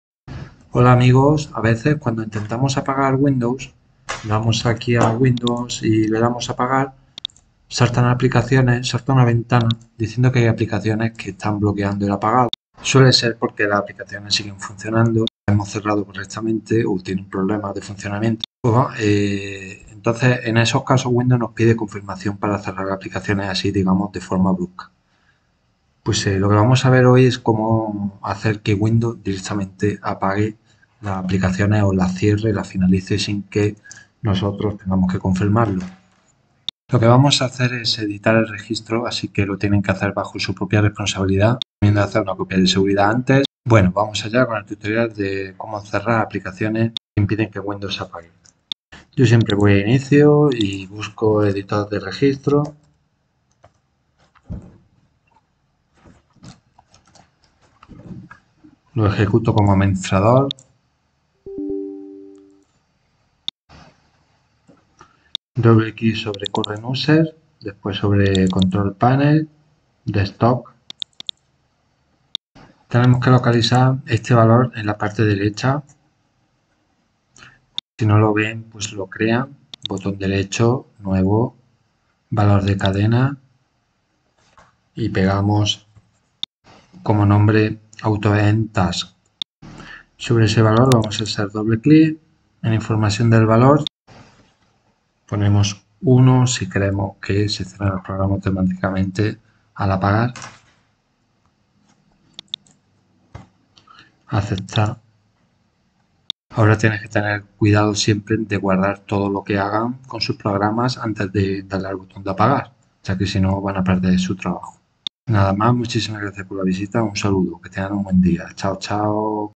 Cómo forzar el cierre de aplicaciones al apagar Windows 11 automáticamente.
Con el valor AutoEndTasks configurado correctamente podemos matar el proceso de aplicaciones que bloqueen el apagado de Windows de forma automática. Te enseño cómo modificar o añadir este valor al editor de registro para auto finalizar aplicaciones al apagar Windows 10 o Windows 11.